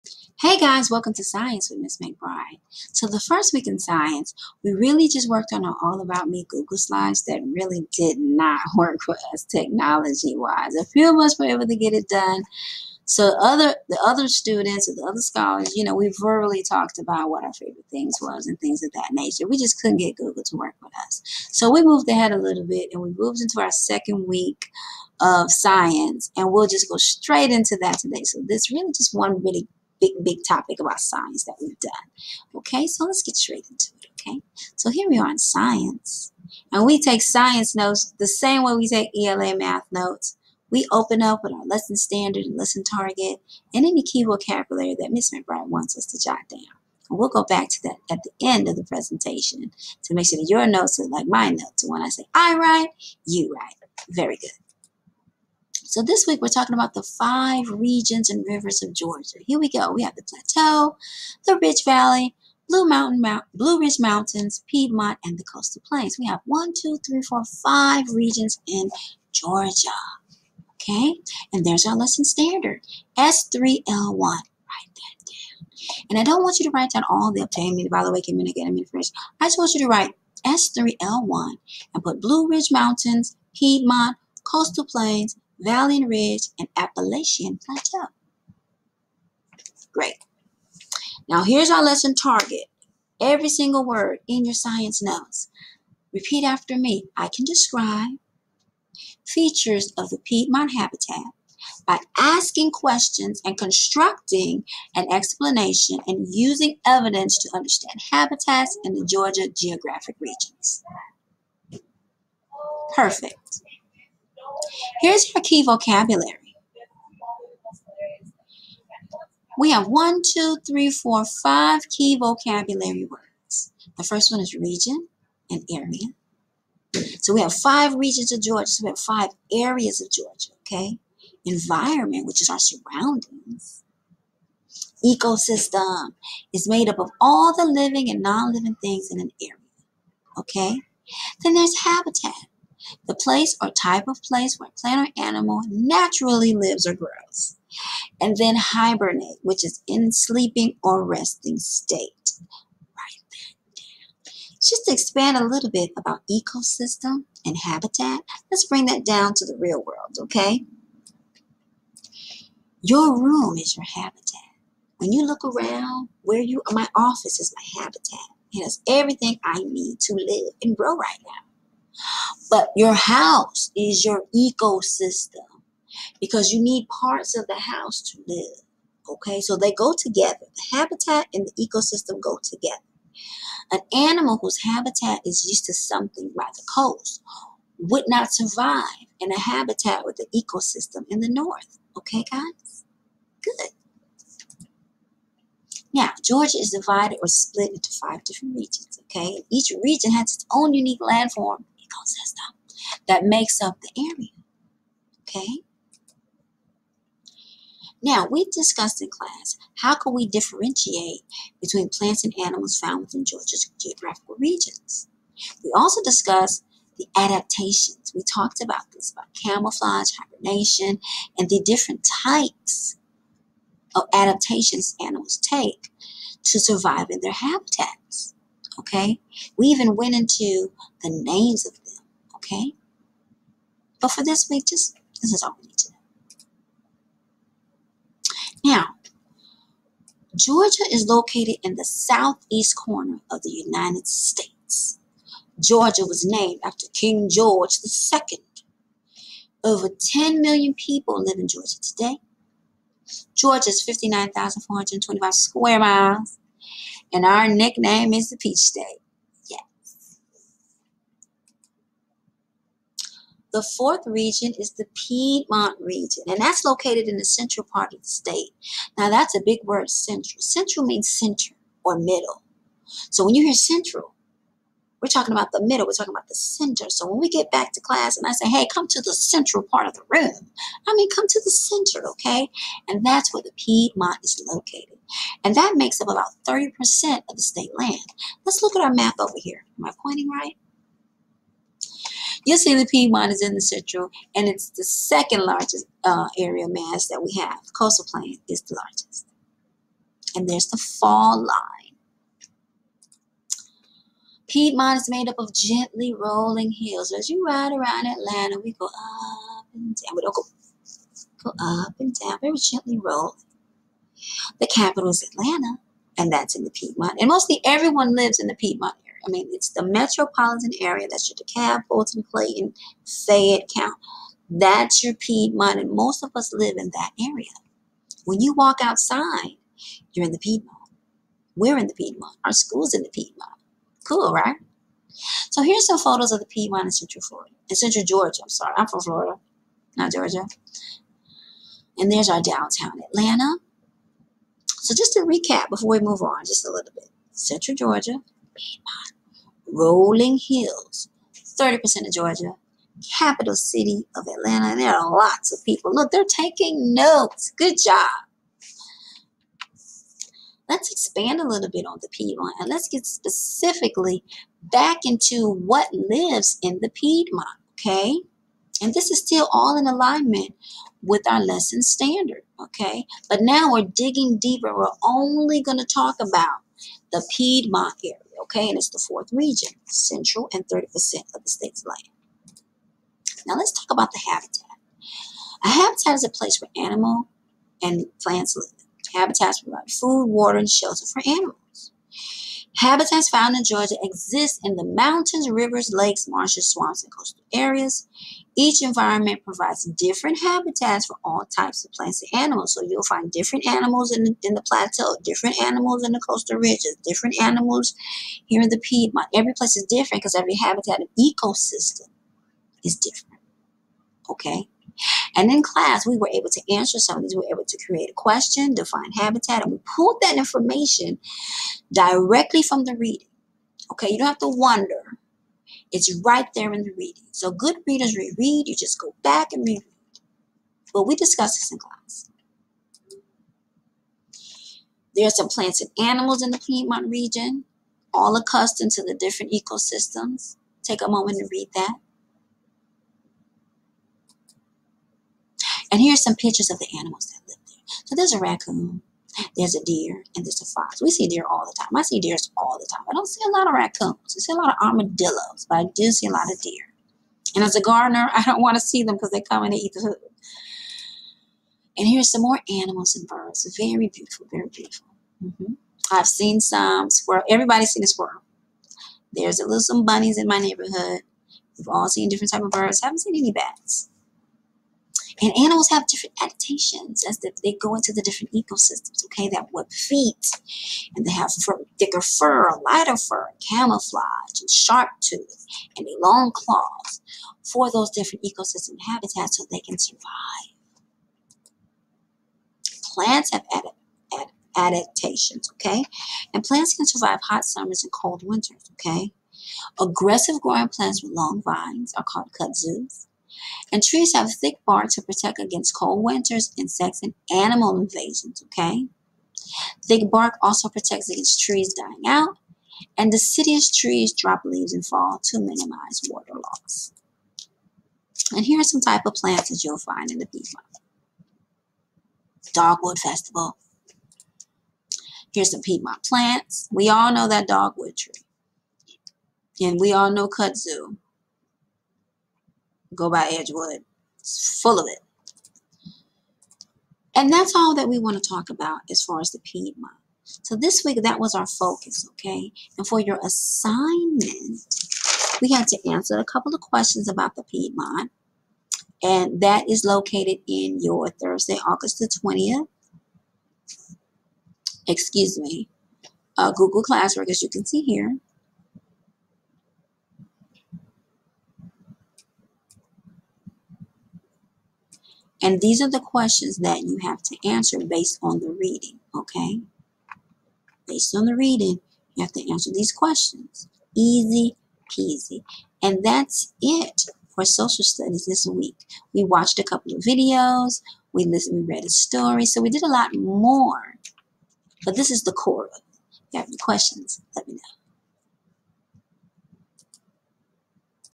Georgia Regions, The Piedmont, Animal and Plant Adaptations
Five Regions of Georgia: The Piedmont, Animal and Plant Adaptations

In this video I will teach you about the The Piedmont, Animal and Plant Adaptations

This is the 3rd grade standard (Georgia).

Here's a rundown of what your going to learn:

1) What are the Georgia Regions.
2) Piedmont Animal Adaptations
3) Animal Adaptations

By the end, you will know all about the The Piedmont, Animal and Plant Adaptations